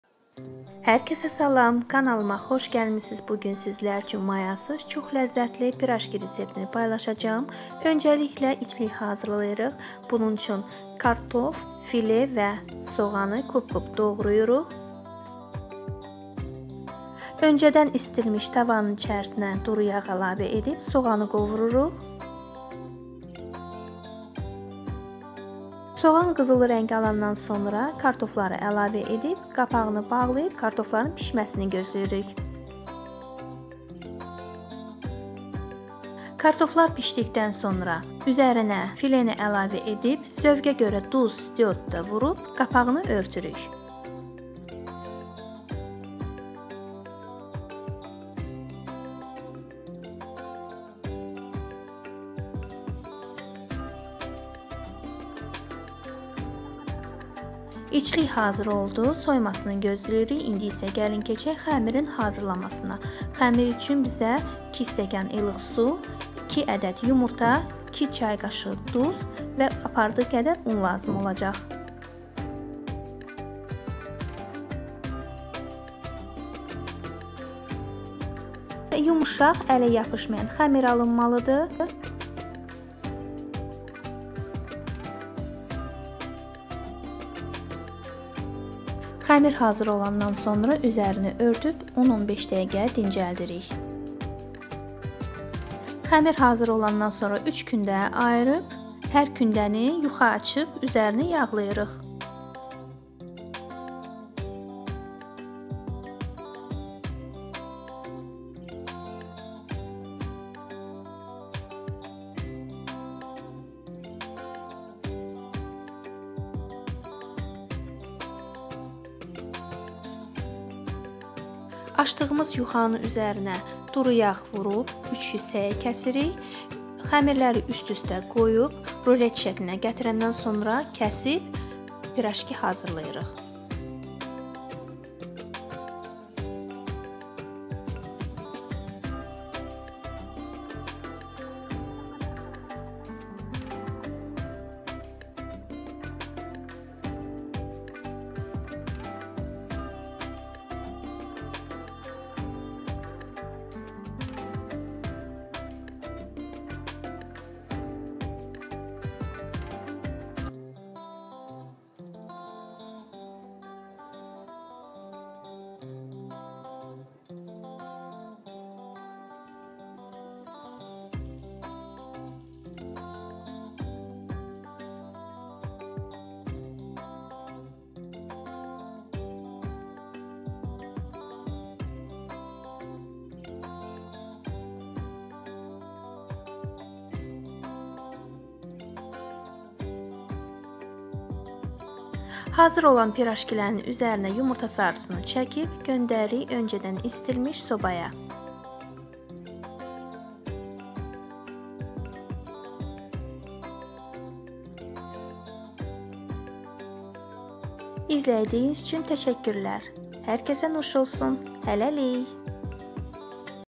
Mayasız qat-qat pirojki | Слоёные пирожки без дрожжей | Yeast-free puff pastry
Mayasız qat-qat pirojki |
Слоёные пирожки без дрожжей |
Yeast-free puff pastry

İçlik üçün:
2 əd kartof
3 əd file
1 əd soğan
0.5 armudu stəkan duru yağ

Xəmiri üçün:
2 st ilıq su
2 ç.q duz
2 əd yumurta
Apardığı gədər un
0.5 st duru yağ
Üzərinə:
2-3 əd yumurta sarısı
Для начинки:
2 шт. картошки
3 шт филе
1 репчатый лук
50 гр растительного масла

Для теста:
2 ст тёплой воды
2 ч.л соли
2 яйца
Мука сколько потребуется
100 гр растительного масла

Для смазывания
2-3 желтка

for the filling:
 2 potatoes
 3 fillets
 1 onion
 50 g vegetable oil

For the test:
 2 tbsp warm water
 2 tsp salt
 2 eggs
 How much flour is needed
 100 g of vegetable oil

 For lubrication
 2-3 yolks